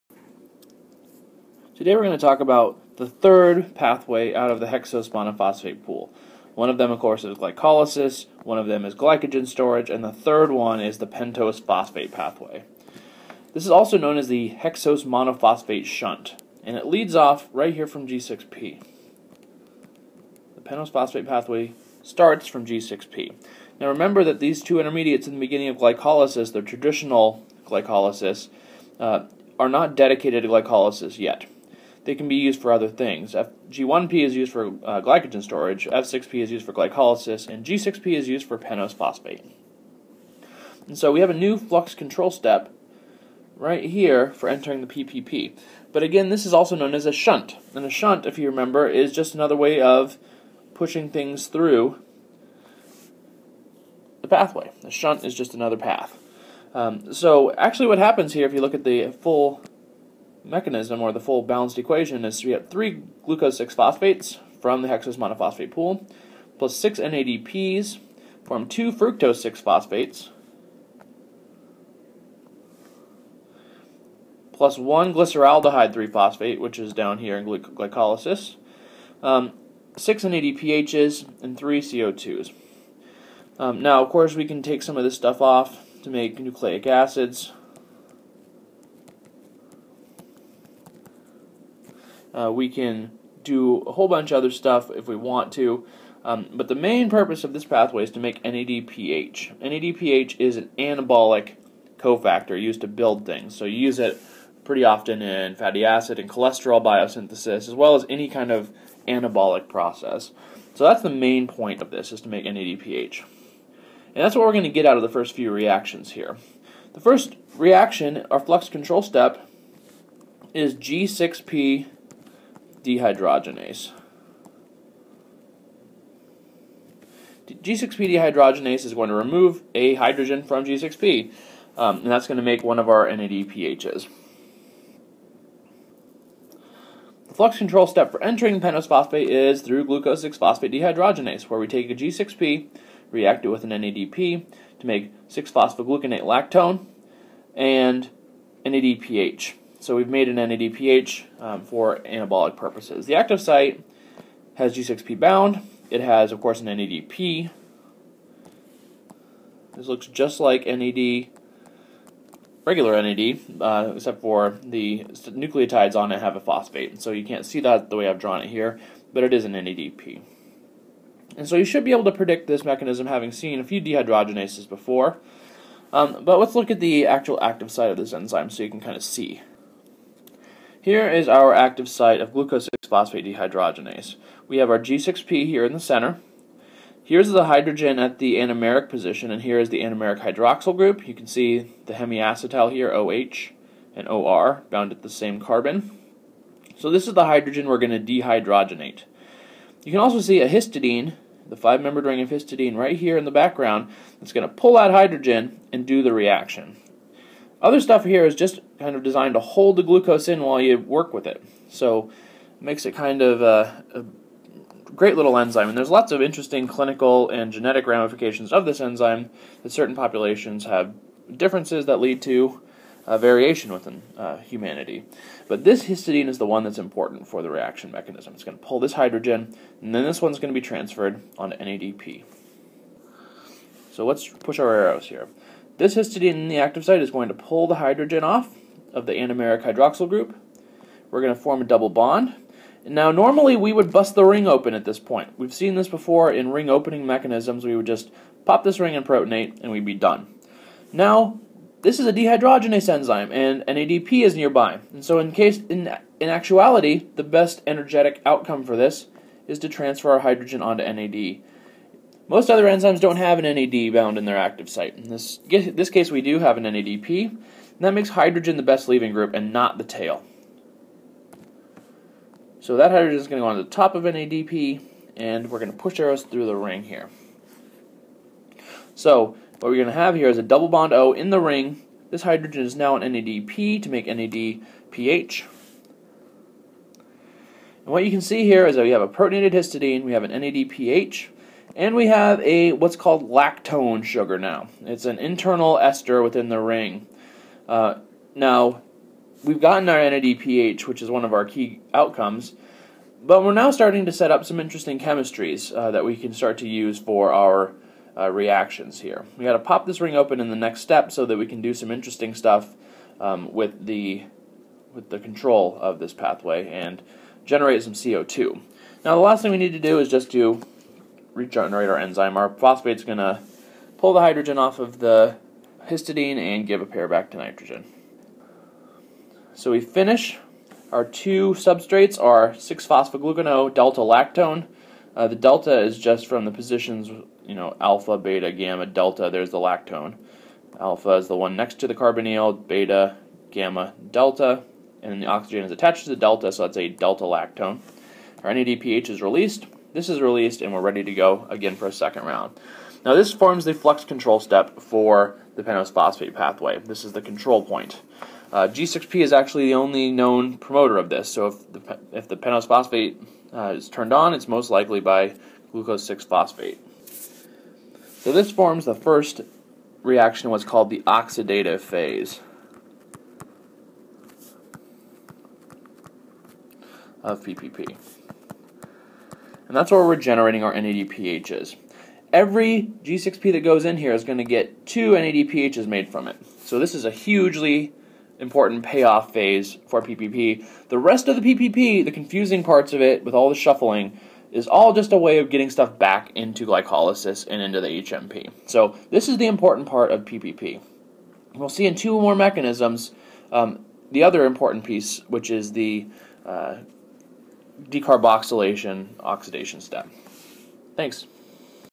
CHEM 407 - Pentose Phosphate Pathway
The flux controlling step of the PPP is the oxidation of G-6-P to 6-phosphoglucono-d-lactone, generating the anabolic substrate NADPH. This is also the first step of the oxidative phase of the PPP